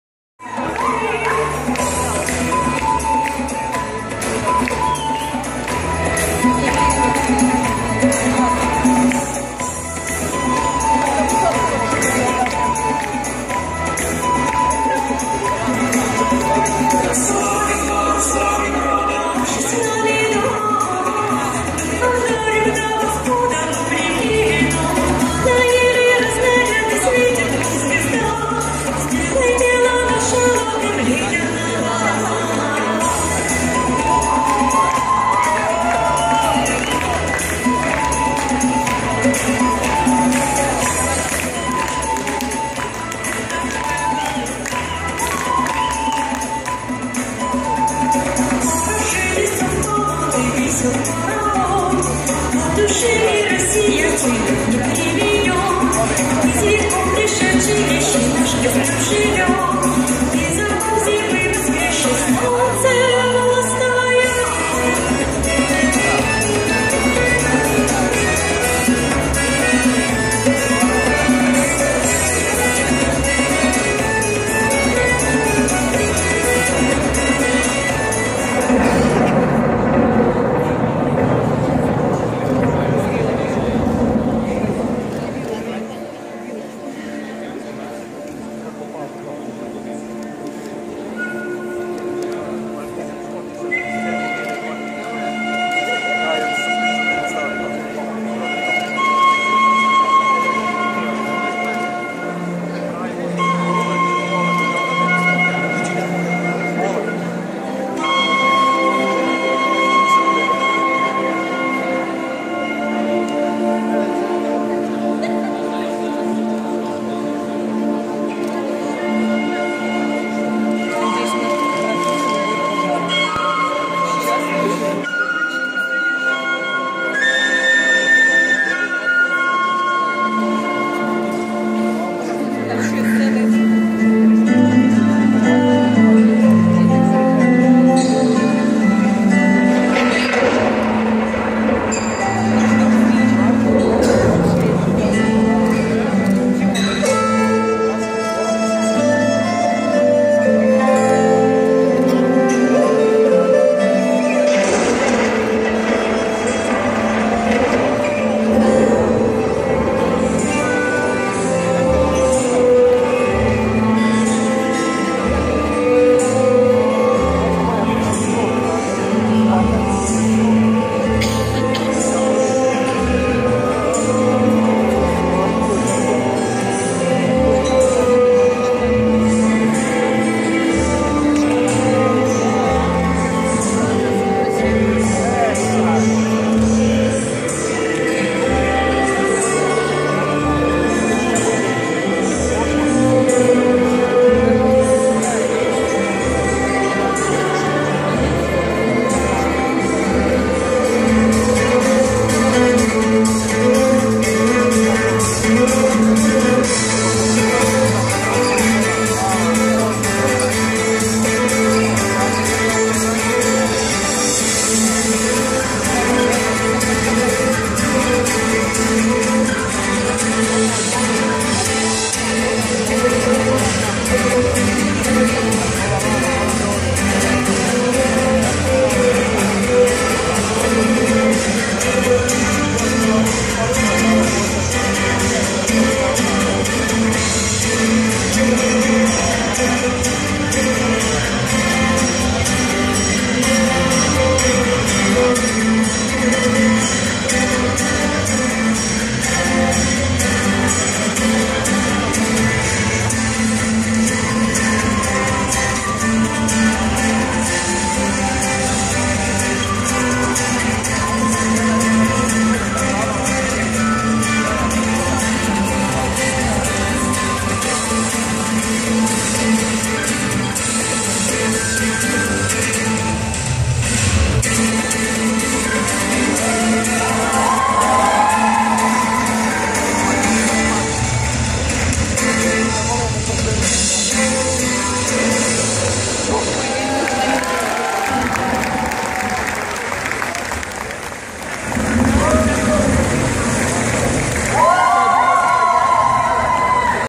Славянский танец Громница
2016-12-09 Международный студенческий фестиваль "Венок народных традиций"